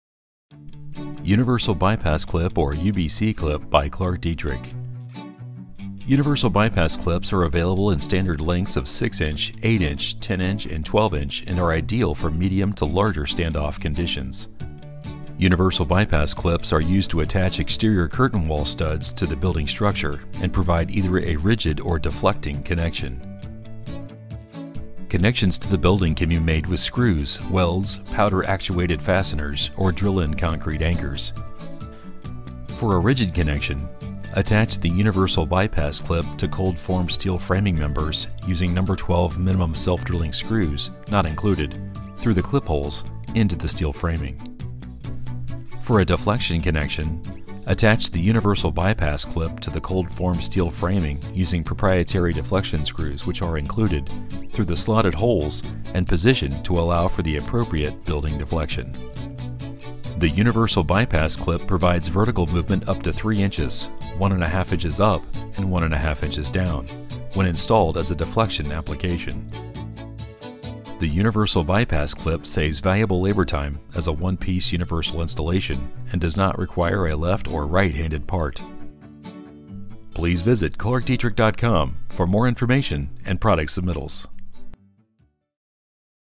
Universal Bypass Clips (UBC)
Provides either a rigid connection OR vertical building movement up to 3”.
Universal Bypass Clips are used to attach exterior curtain wall studs to the building structure and provide either a rigid connection or deflecting connection for vertical building movement independent of the cold-formed steel framing.